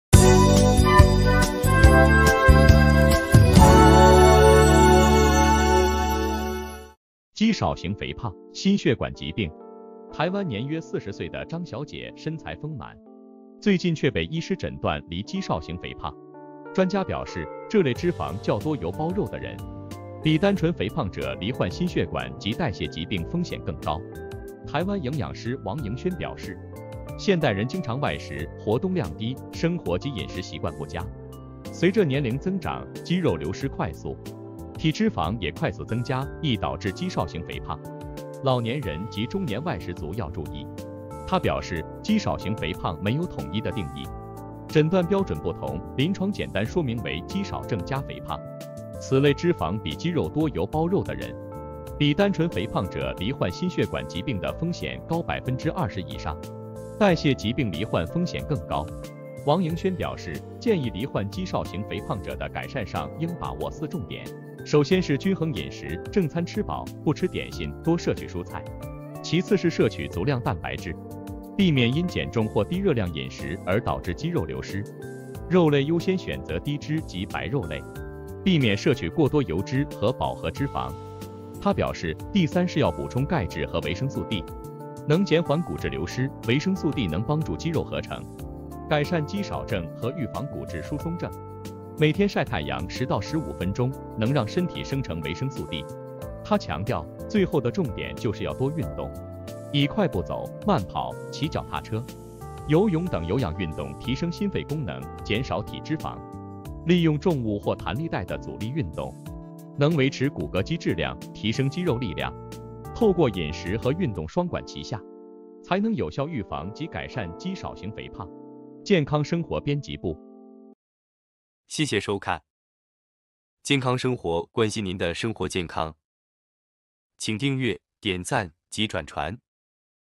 肌少型肥胖 心血管疾病
2024年6月26日

台灣年約40歲的張小姐身材豐滿，最近卻被醫師診斷罹「肌少型肥胖」。專家表示，這類脂肪較多「油包肉」的人，比單純肥胖者罹患心血管及代謝疾病風險更高。
台灣營養師王盈媗表示，現代人經常外食、活動量低，生活及飲食習慣不佳，隨著年齡增長，肌肉流失快速，體脂肪也快速增加，易導致「肌少型肥胖」，老年人及中年外食族要注意。
她表示，「肌少型肥胖」沒有統一的定義，診斷標準不同，臨床簡單說明為「肌少症加肥胖」，此類脂肪比肌肉多「油包肉」的人，比單純肥胖者罹患心血管疾病的風險高20%以上，代謝疾病罹患風險更高。
王盈媗表示，建議罹患肌少型肥胖者的改善上應把握4重點，首先是均衡飲食，正餐吃飽、不吃點心，多攝取蔬菜。其次是攝取足量蛋白質，避免因減重或低熱量飲食而導致肌肉流失。肉類優先選擇低脂及白肉類，避免攝取過多油脂和飽和脂肪。
她表示，第3是要補充鈣質和維生素D，能減緩骨質流失，維生素D能幫助肌肉合成，改善肌少症和預防骨質疏鬆症。每天曬太陽10到15分鐘，能讓身體生成維生素D。
她強調，最後的重點就是要多運動，以快步走、慢跑、騎腳踏車、游泳等有氧運動提升心肺功能、減少體脂肪；利用重物或彈力帶的阻力運動，能維持骨骼肌質量、提升肌肉力量。透過飲食和運動雙管齊下，才能有效預防及改善肌少型肥胖。